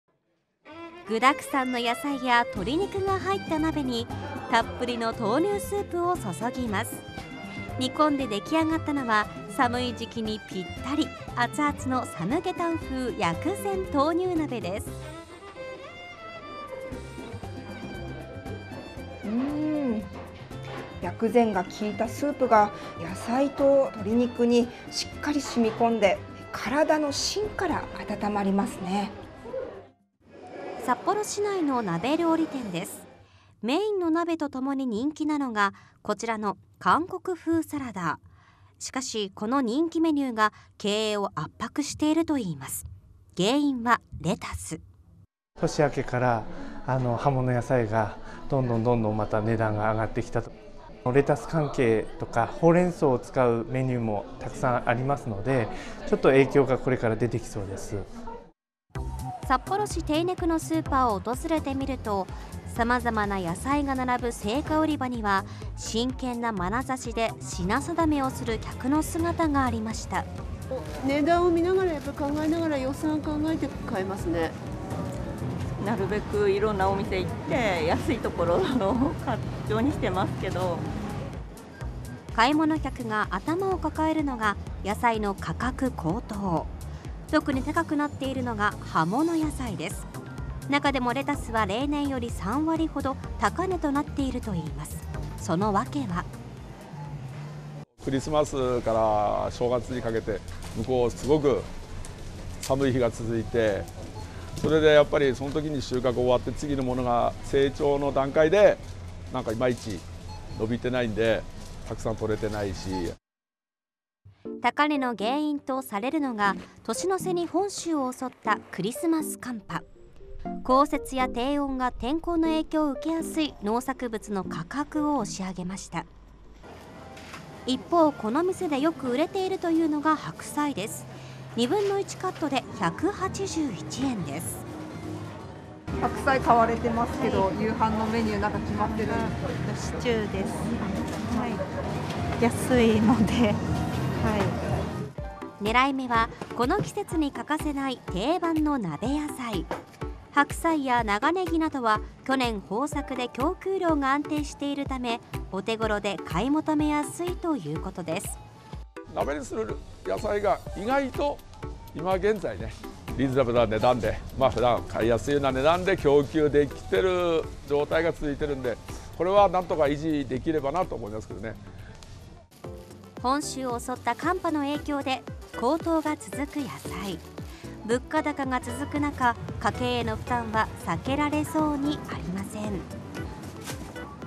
【葉物野菜が高騰】レタスは例年より３割高値 鍋物野菜の白菜や長ネギはお手頃価格に
スーパーなどで「高いなあ」と感じていらっしゃる方も多いのではないでしょうか。サラダに欠かせないレタスなどの葉物野菜が、いま高騰しています。物価高が続くなか、負担は避けられそうにありません。（2023年1月12日放送）